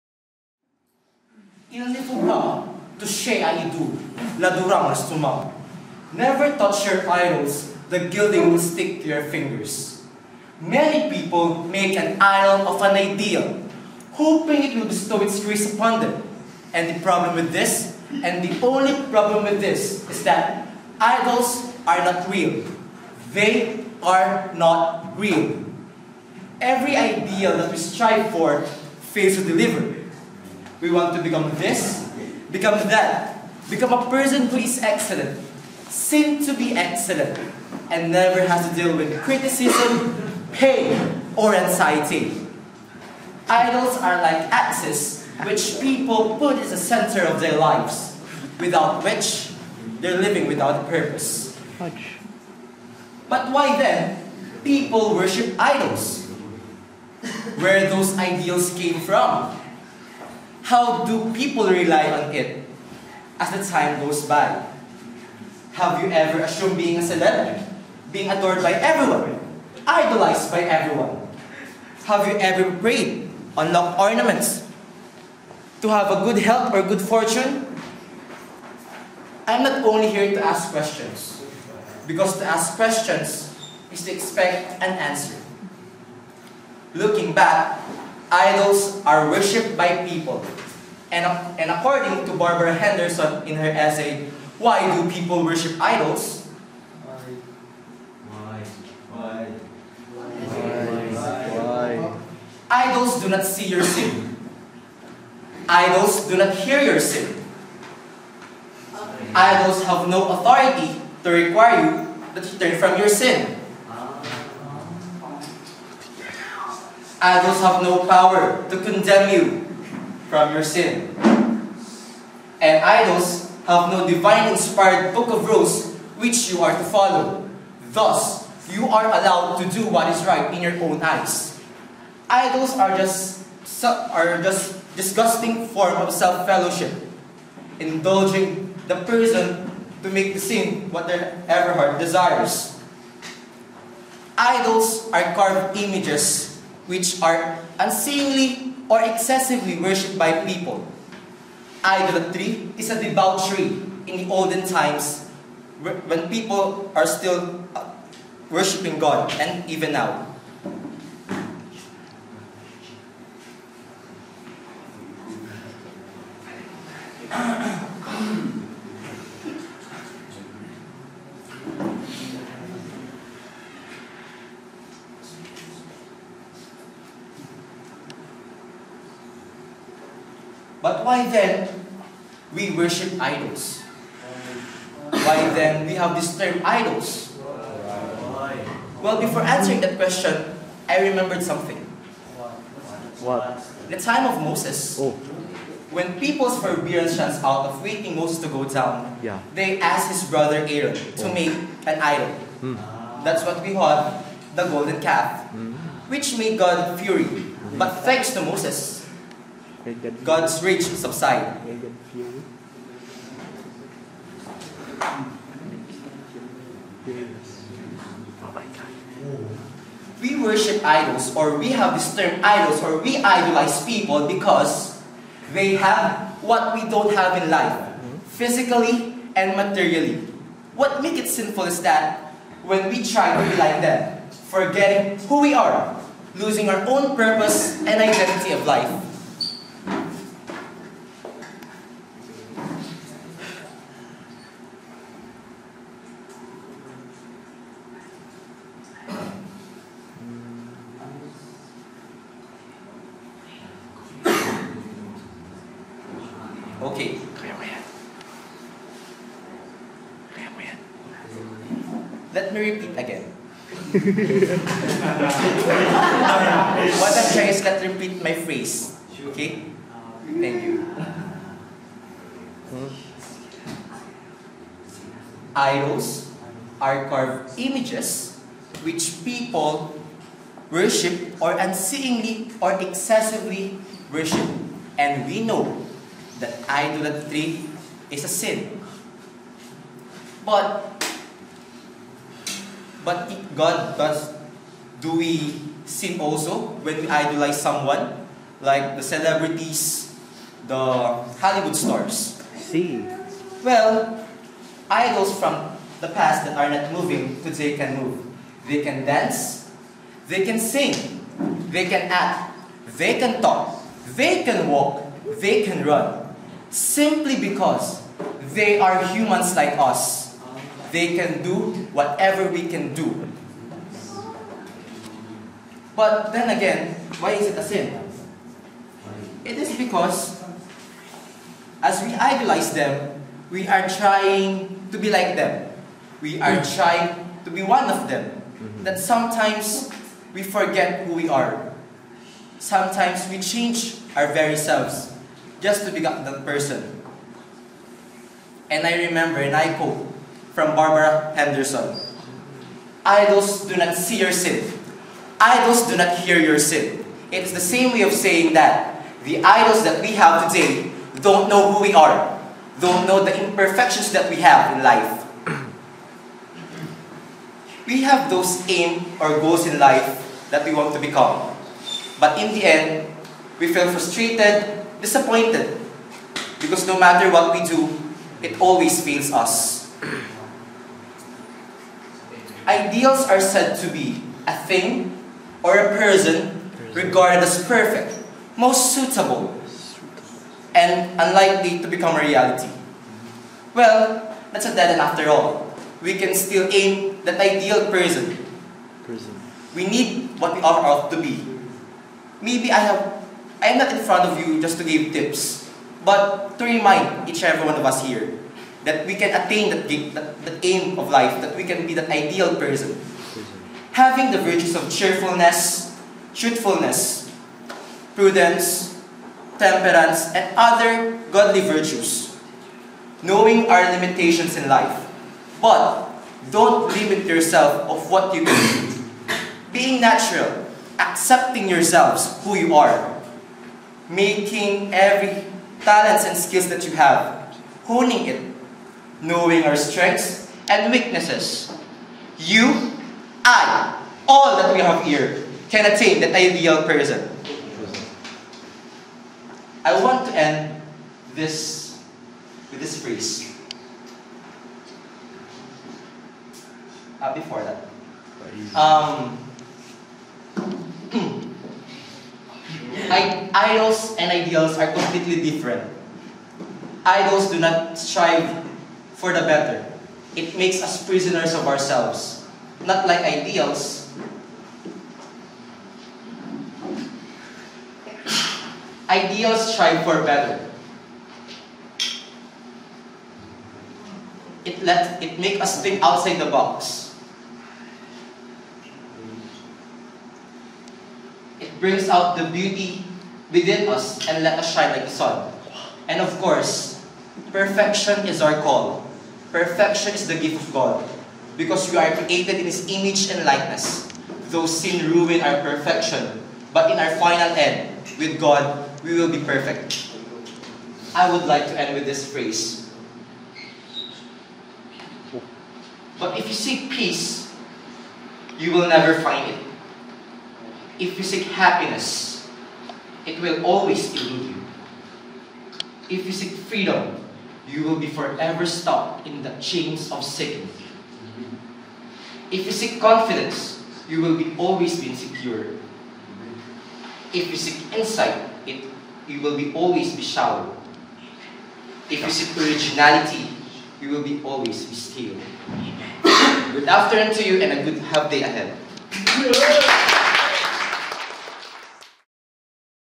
Speech 04 140305 Noel
Speech Class Finals: speeches delivered by Claretian students and externs of St. Anthony Mary Claret College, QC, Philippines, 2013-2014.